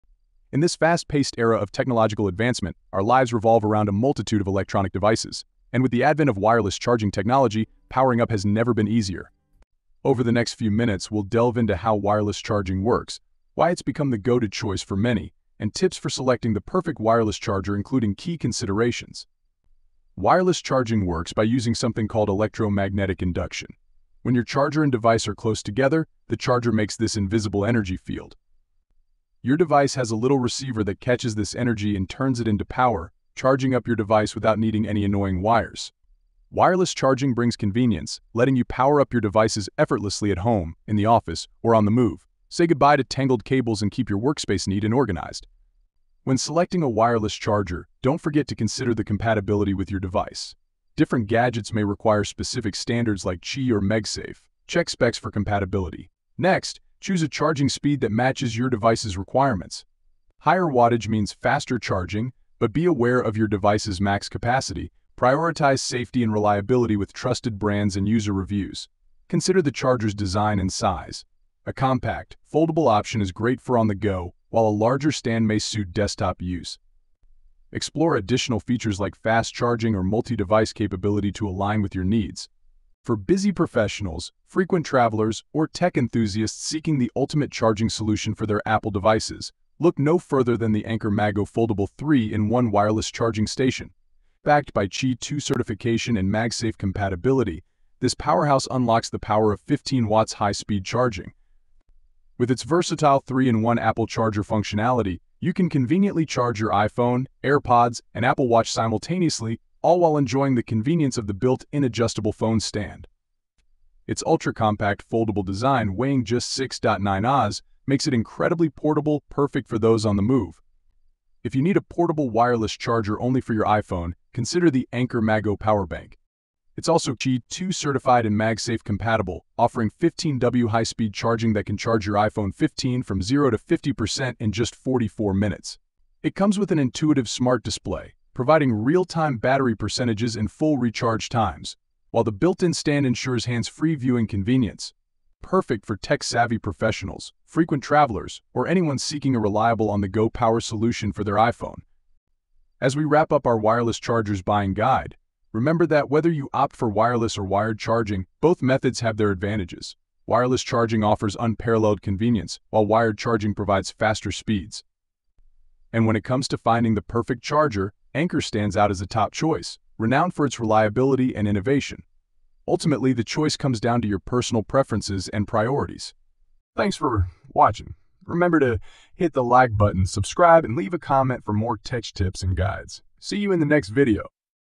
Wireless Chargers - Everything to Know Before You Buy
Discover the world of wireless charging in our new YouTube video. We break down the technology, benefits, and various types of wireless chargers, guiding you on factors to consider such as compatibility, speed, design, and cost. We also offer top recommendations across different categories. The video ends with a comparison of wireless and wired charging, outlining pros and cons to aid your decision-making process.